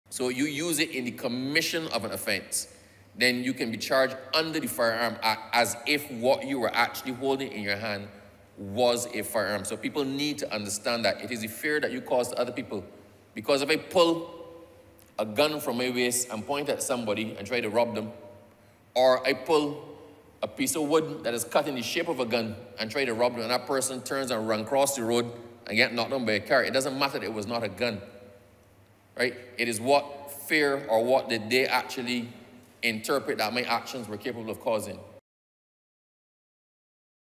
New laws to cover imitation guns
The Firearms (Amendment) Bill 2022 revises the definition of imitation firearms.

Speaking in the House of Assembly today, Home Affairs Minister Wilfred Abrahams, explained the term now covers anything that appears to be a firearm, whether or not it's capable of discharging shots.